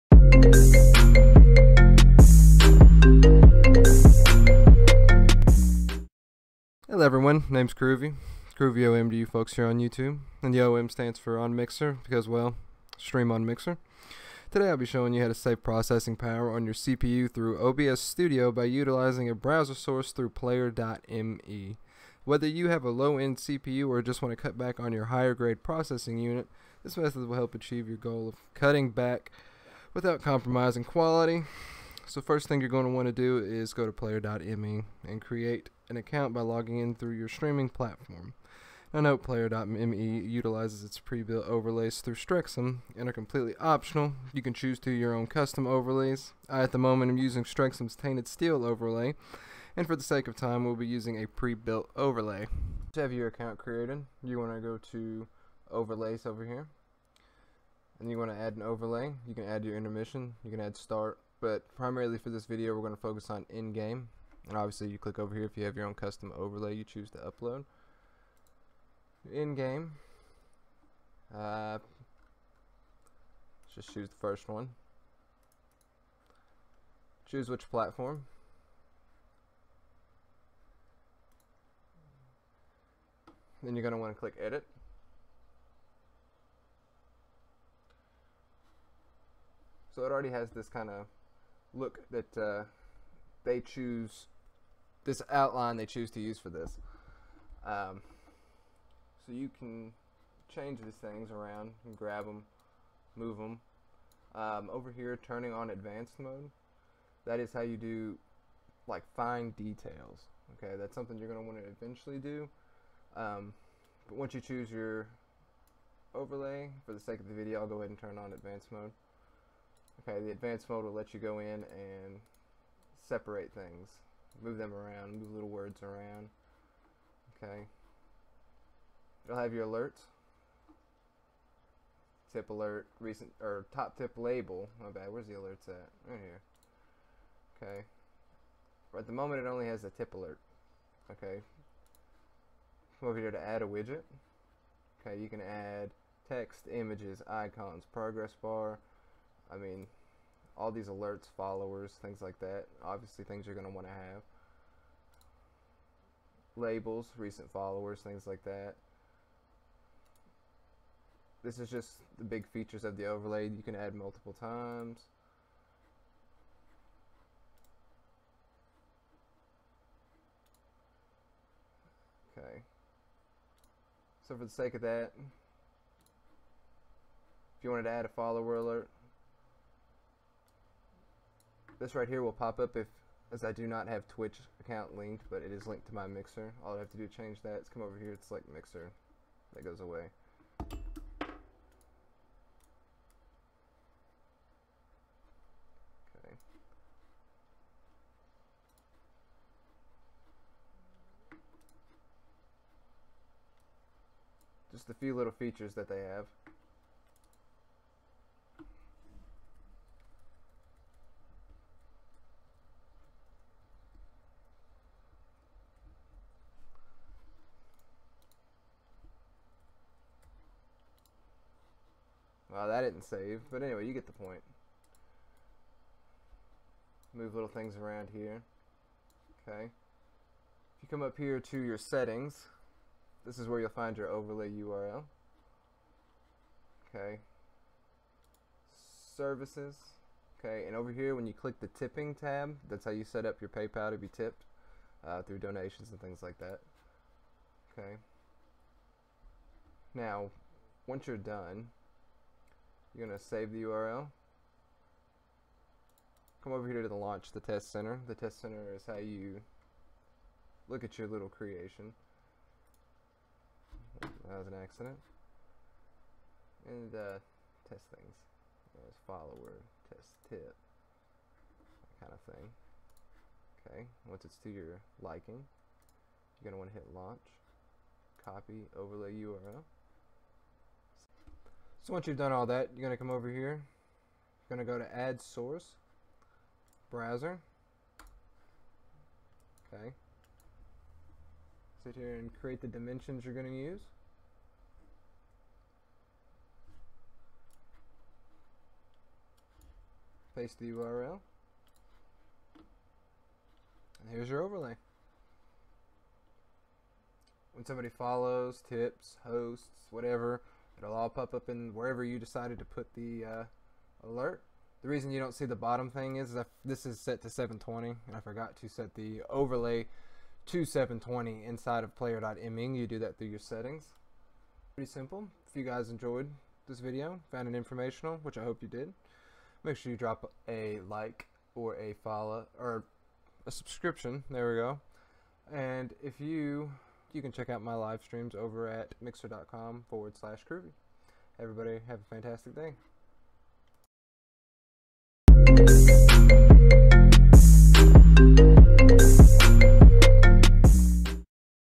Use less CPU with Overlays, Alerts & Widgets!
How to use less processing power from your CPU to run a nice overlay, follower/host/sub alerts, and tons of widgets on Twitch or Mixer! Whether you're wanting to save CPU because your CPU is low-end or just to cut back even with a high-grade CPU, this method will help you!
Xbox: Kroovy on Mixer